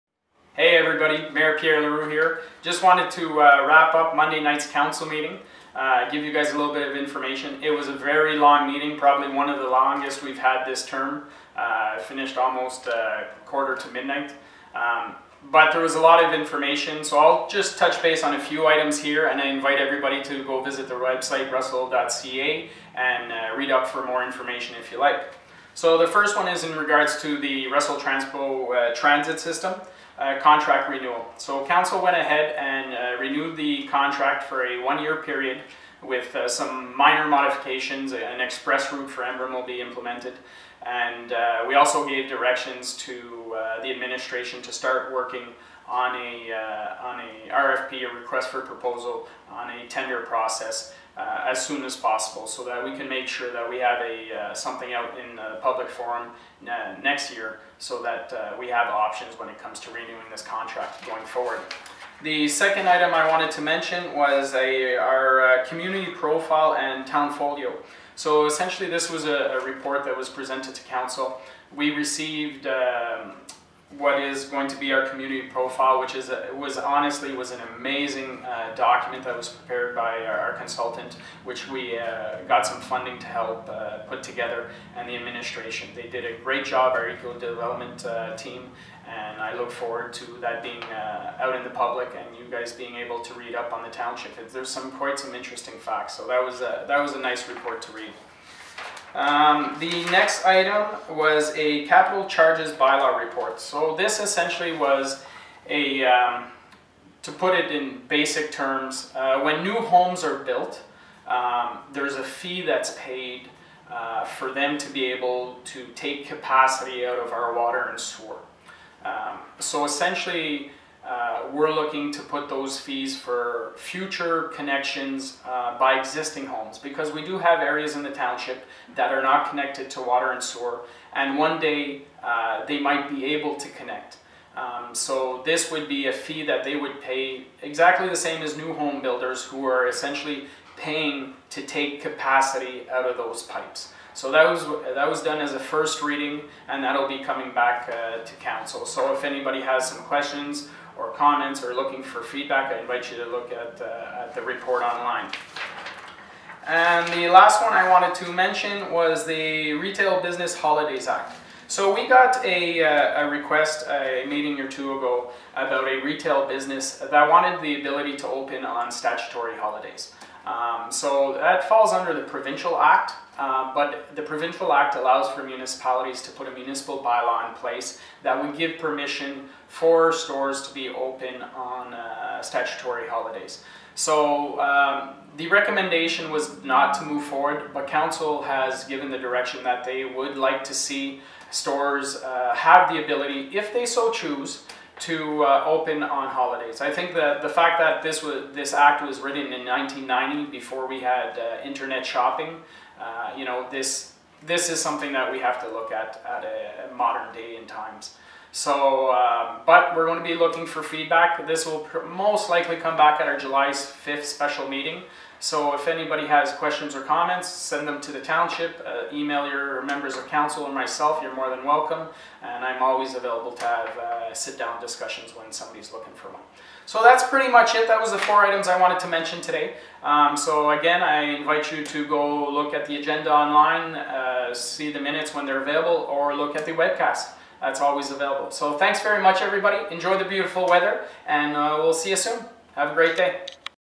June 18, 2018 – Council Meeting Wrap-Up
1. Russell Transpo Public Transportation Contract - Report ECO-DEV 03-2018
2. Community Profile & Townfolio - Report ECO-DEV 04-2018
3. Capital Charges By-law- Report FS 22-2018
4. Retail Business Holiday Act - Report PIC 19-2018